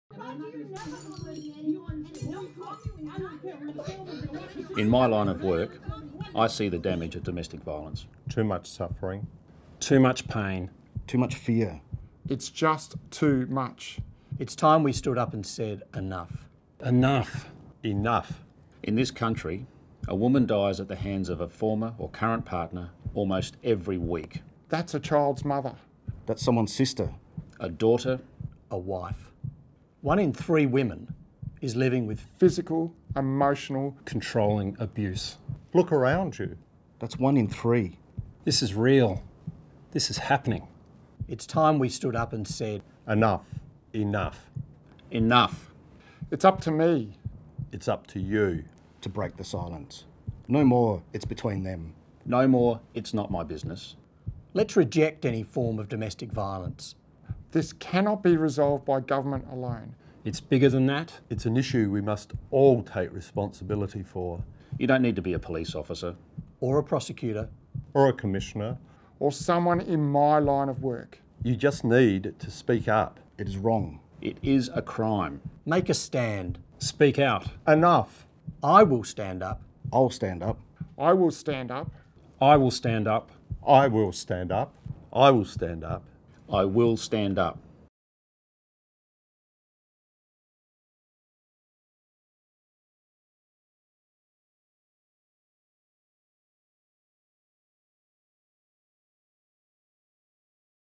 iwillstandup - Stop Domestic Violence
Family violence is a scourge that affects us all. The vast majority of domestic or family violence and sexual assault is perpetrated by men against women. And while the form of violence, its severity and its impact may vary, people working in the justice system know all too well the shocking toll that domestic violence can have on individuals and how it can tear families apart.

Senior leaders in the South Australian justice system are challenging those who excuse, justify or condone violence and inequality, or who turn a blind eye. For the sake of those who have been abused, their families and the broader community, we are asking you to say IWillStandUp and start a behavioural and social change and to stop domestic violence.

Even though a minority of men actually perpetrate violence against women, many men are often silent in the face of violence, sexual assault or attitudes which excuse or minimise violence and control in relationships.

To report any information about domestic violence, call Crime Stoppers on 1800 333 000 or use the Crime Stoppers online reporting

Information will be treated in the strictest of confidence. Please do not report any crimes using social media. Call 000 in an emergency.

Produced by the South Australian Attorney-General’s Department, in conjunction with South Australia Police, the Commissioner for Victims’ Rights, the Department for Correctional Services, Consumer and Business Services, the Crown Solicitor’s Office and the Director of Public Prosecutions.